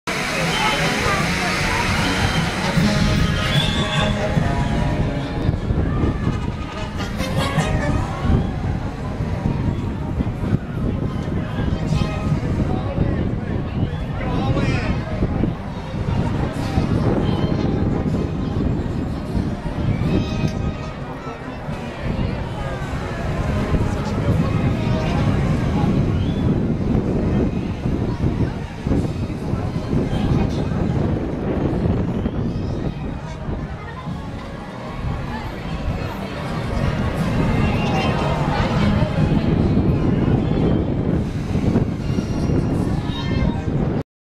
Fantasyland Construction from Dumbo, Walt Disney World 8/22/2010
A look at the Fantasyland construction from Dumbo!